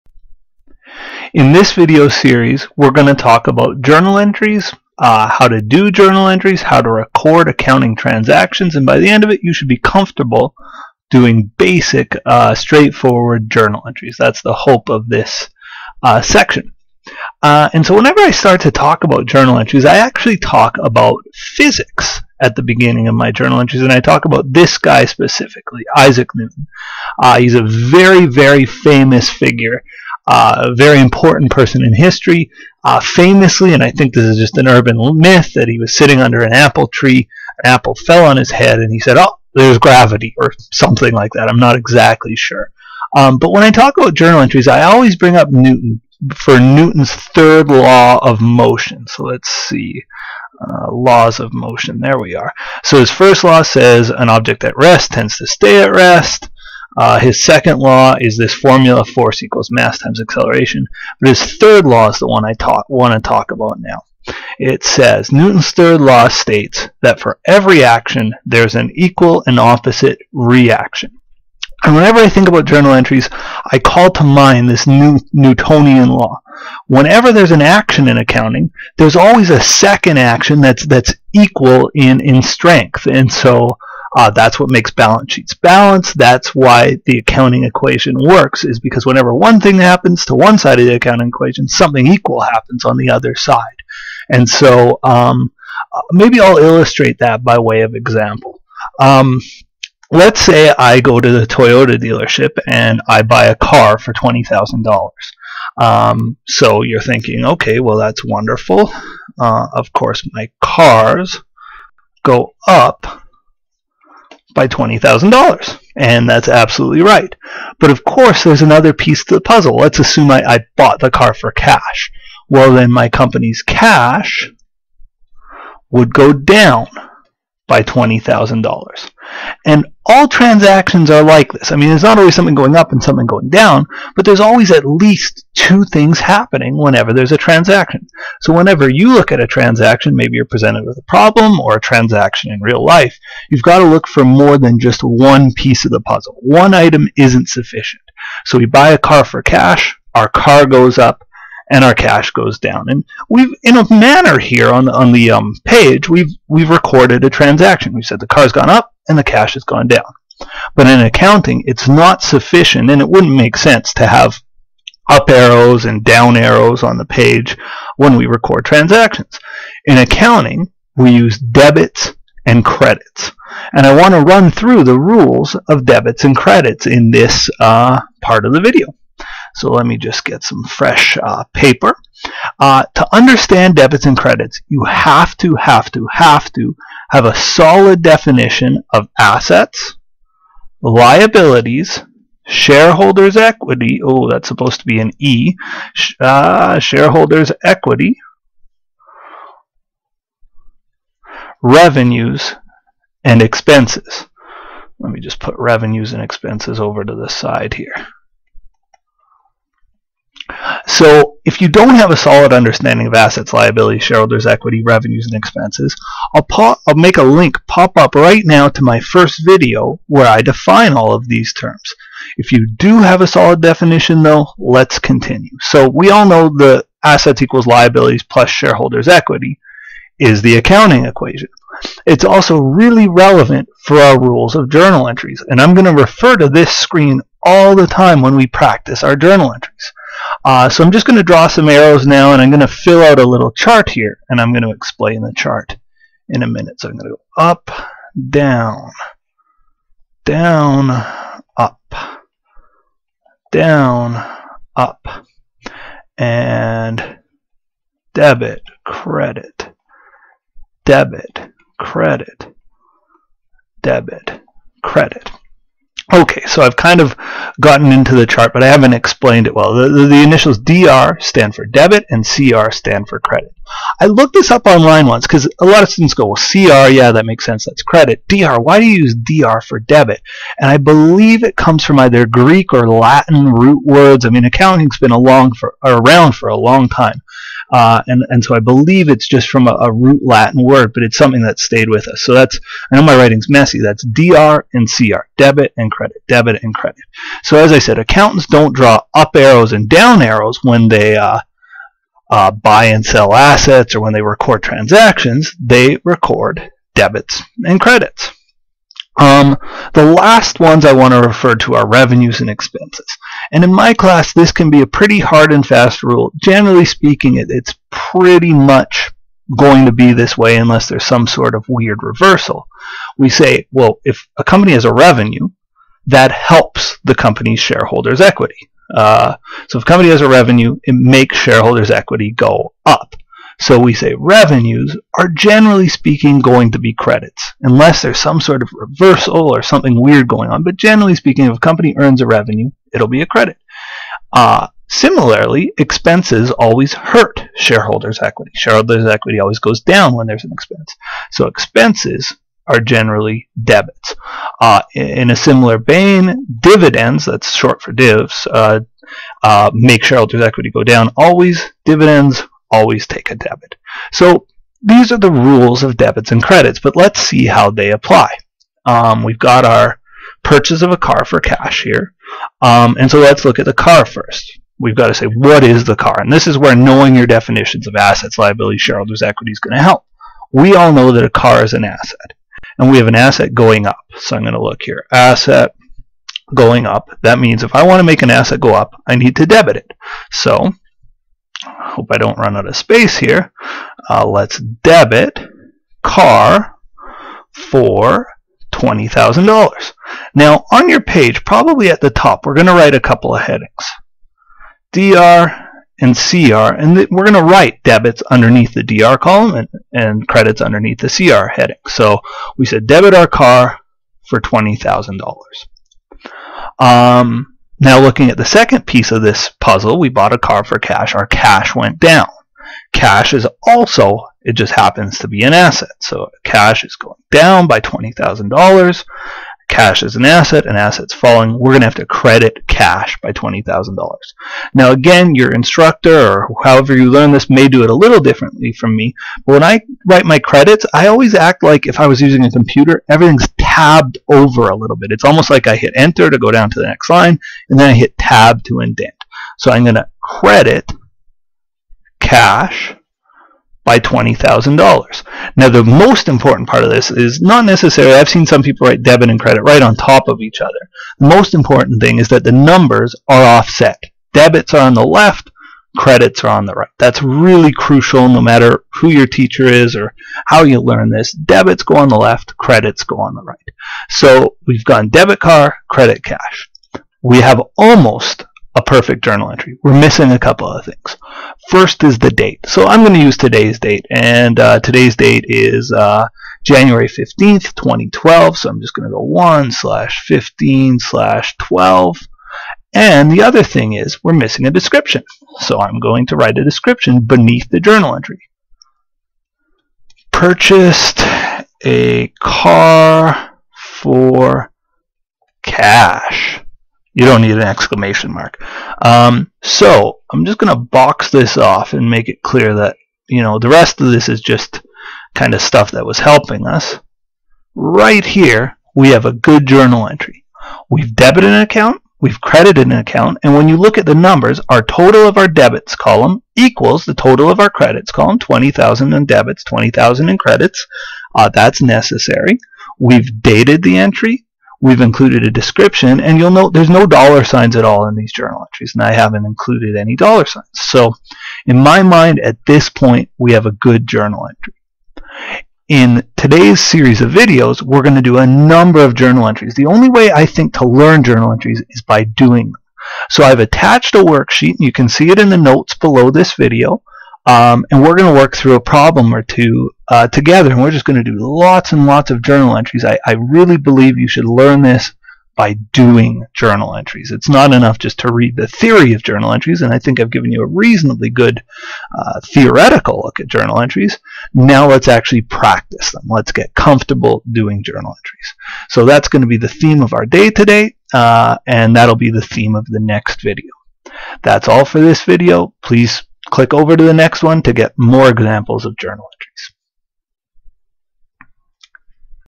Accounting - Unit 2 - Part 1 - Journal Entries - Rules of Debits and Credits
In this video series viewers will learn to prepare basic journal entries.

This video and the attached worksheet were prepared by Tony Bell of Thompson Rivers University - I encourage educators to freely use, edit and modify these videos and the attached worksheet - they are available under Creative Commons Licenses.